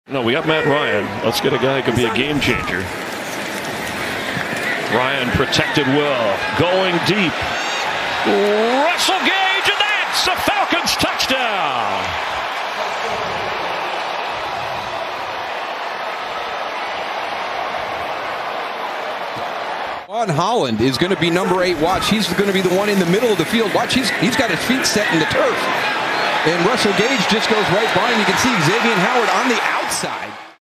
Russell Gage Jr. Catches Deep Ball for Touchdown
Matt Ryan bombs one down to the end zone and finds Russell Gage waiting with open arms for a Falcons score. The Atlanta Falcons take on the Miami Dolphins during Week 7 of the 2021 NFL season.